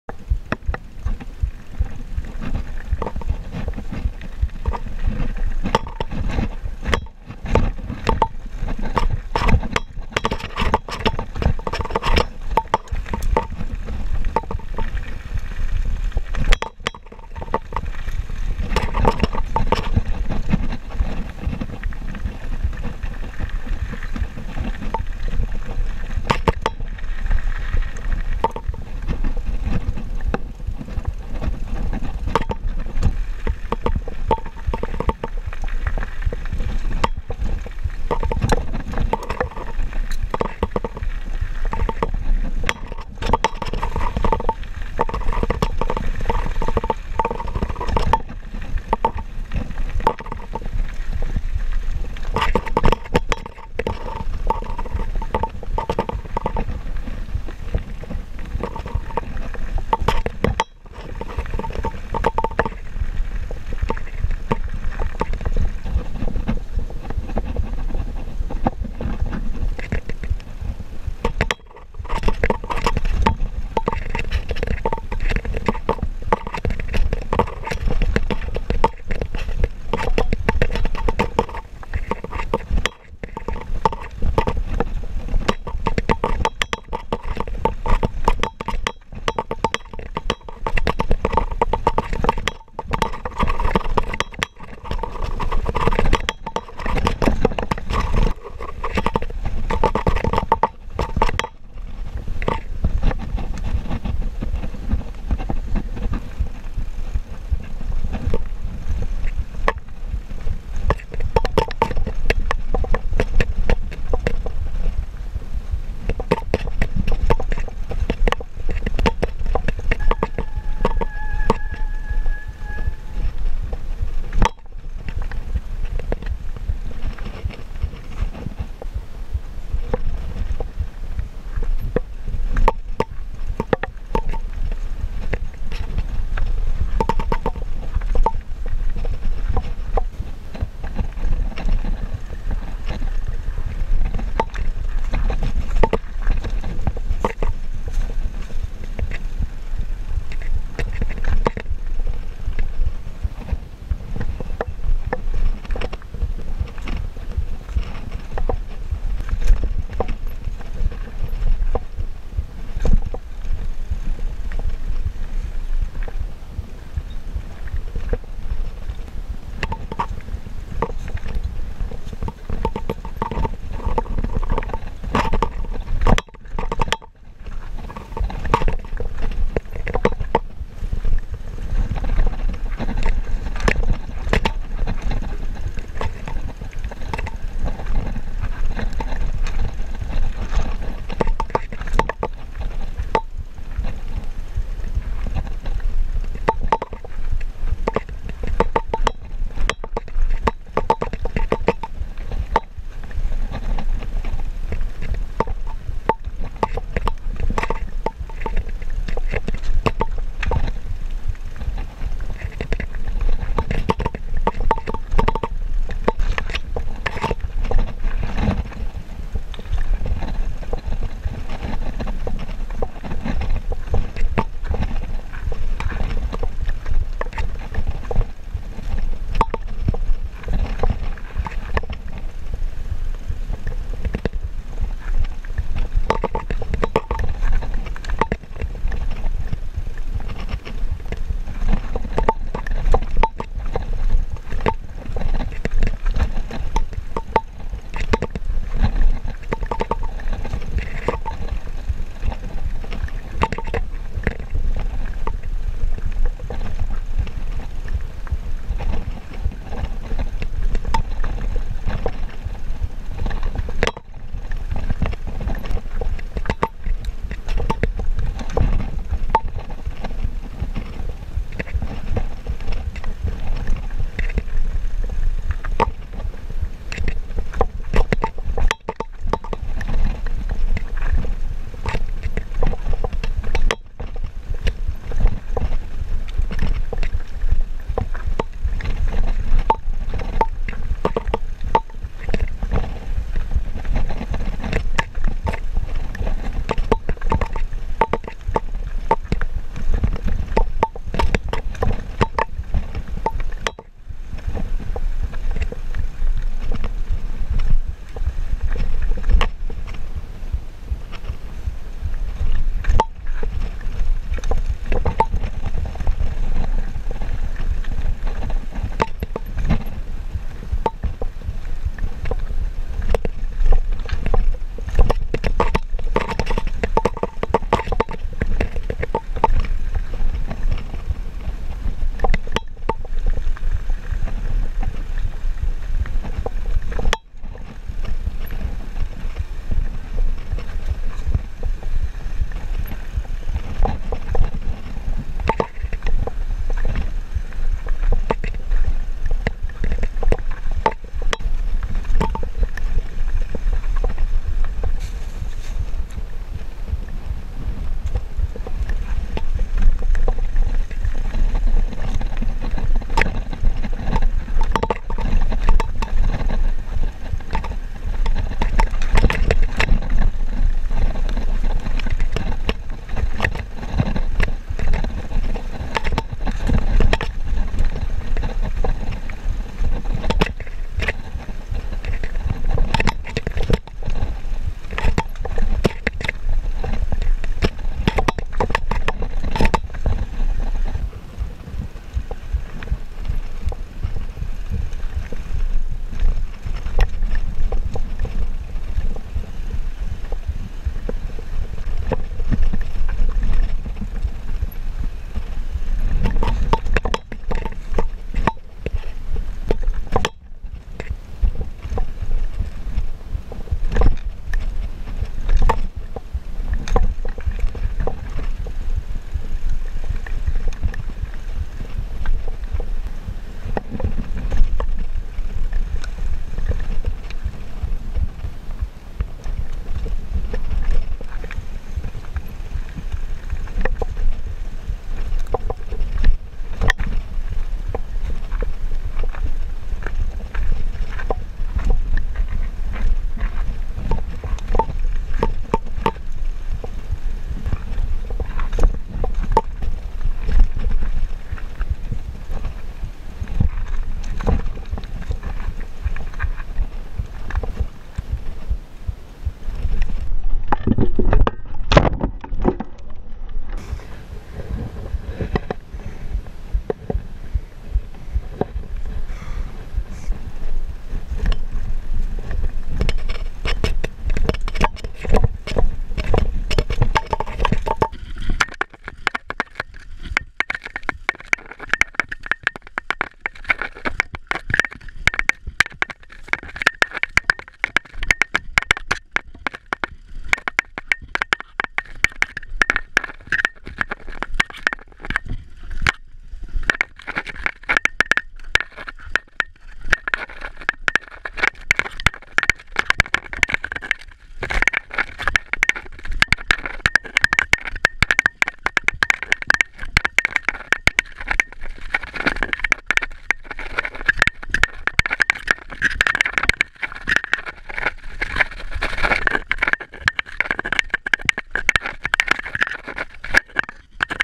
Geiersteintrail
Vom Wanderparkplatz Hohenburg (720m) nach Osten auf dem Fahrweg entlang des Hirschbachs Richtung Hirschtalsattel bzw. Lenggrieser Hütte. Am Hirschtalsattel (1227m) rechts weiter. Später auf 1320m dem Weg nach links Richtung Rauhalm folgen. Oberhalb der Rauhalm auf dem Gratrücken zum Gipfel des Seekarkreuz (1601m). Die Aussicht ist einfach HAMMERmäßig! Den Gipfel in südlicher Richtung überschreiten. Am Weidezaun unterhalb dann auf dem Trail (S1) nach Westen zur Lenggrieser Hütte (1336m) abfahren. Ab der Lenggrieser Hütte auf der Forststrasse ins Tal, auf 1020m rechts ab zum Geierstein...